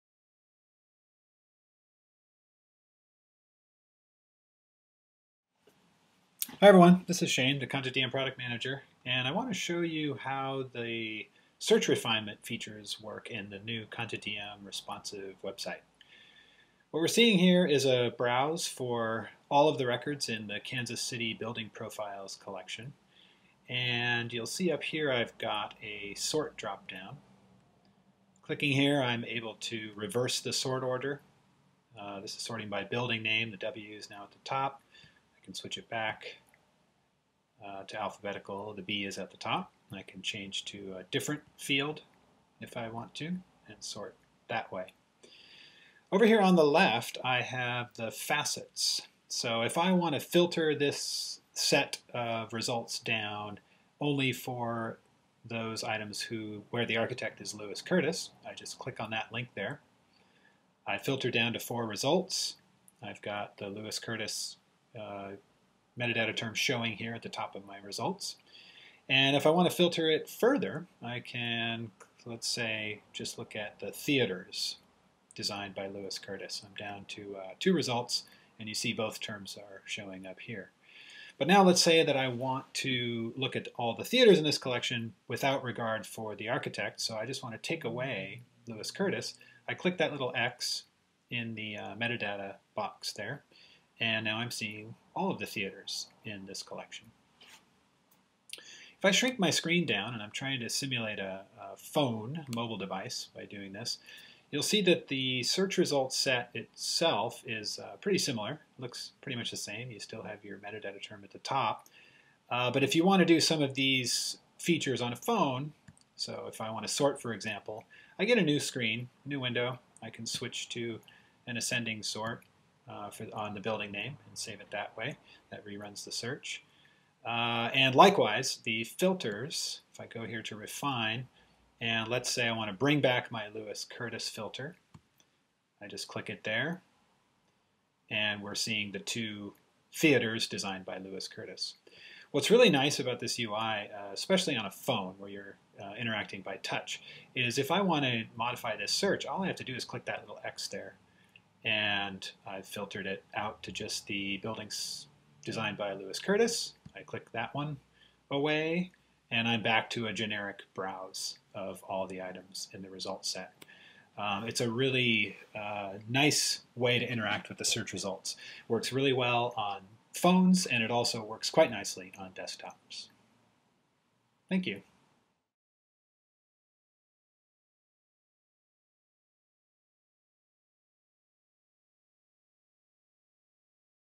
Search refinement features in the CONTENTdm responsive website
Learn more about the many options for search refinement in the CONTENTdm responsive website.